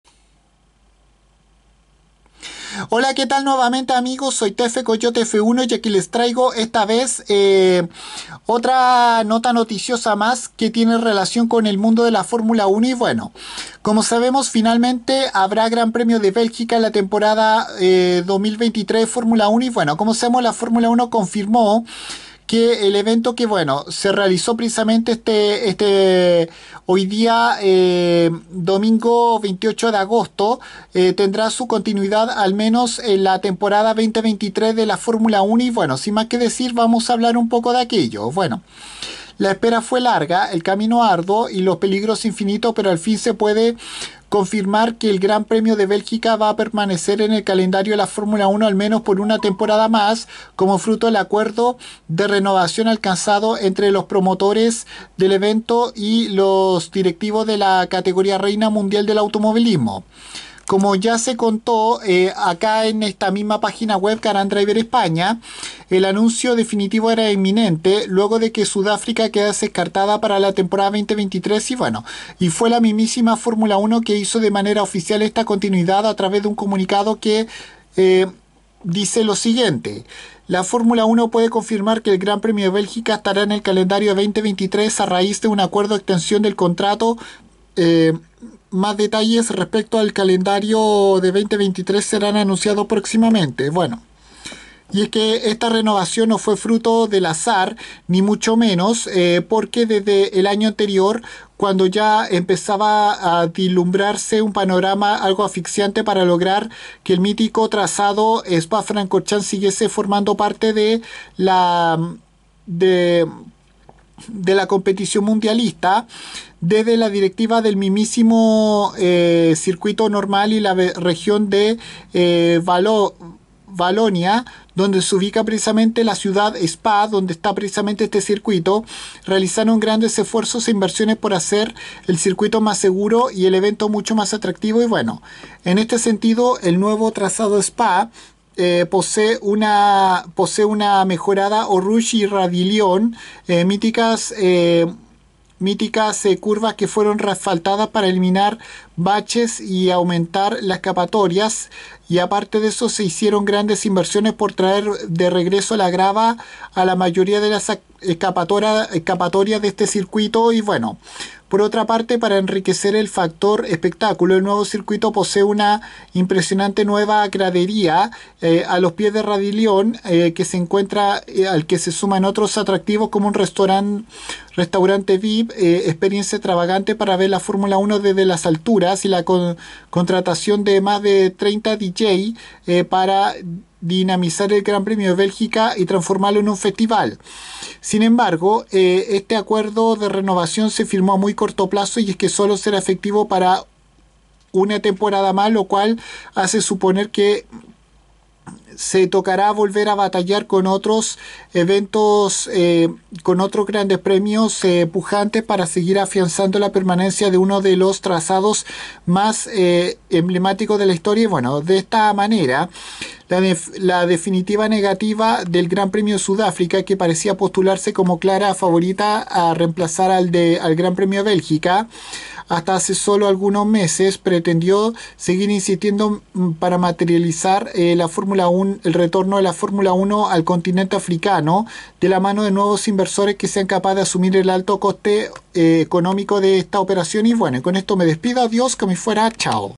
Finalmente habrá GP de Bélgica de F1 en la Temporada 2023
Luego de muchos rumores sobre su posible salida del calendario de la Fórmula 1 el Gran Premio de Spa Francorchamps estará por una temporada mas de cara a la siguiente Temporada 2023